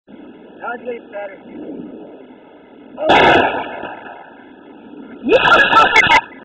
Mini H-Bomb.
Take a water bottle. put tin foil in it. then put The Works in it shake it and watch it work its magic!